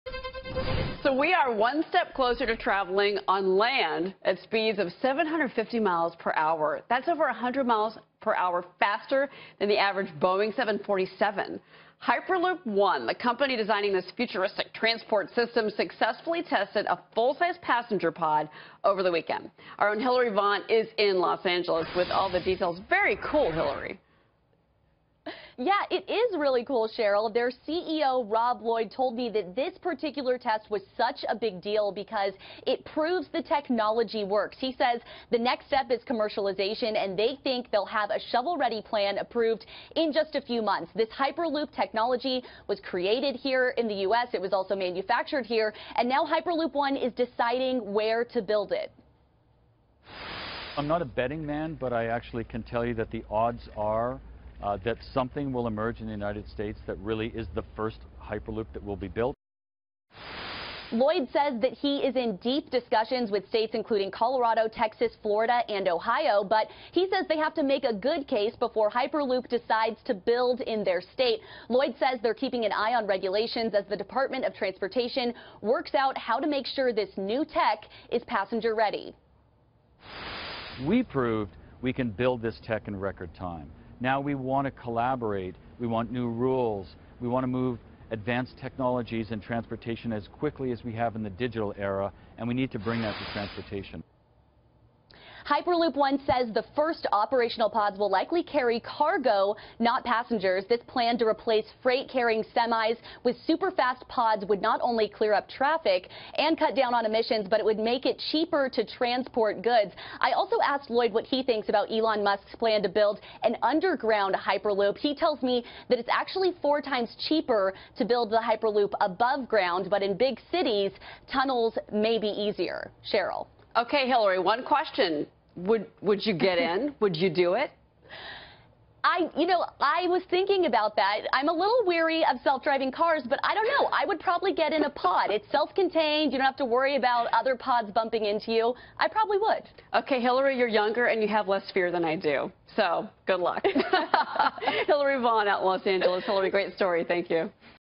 Hyperloop One test successful, now seeking government approval
FBN's Hillary Vaughn speaks with Hyperloop CEO Rob Lloyd, on Hyperloop One's successful test run and the possibility of government approval in a few months.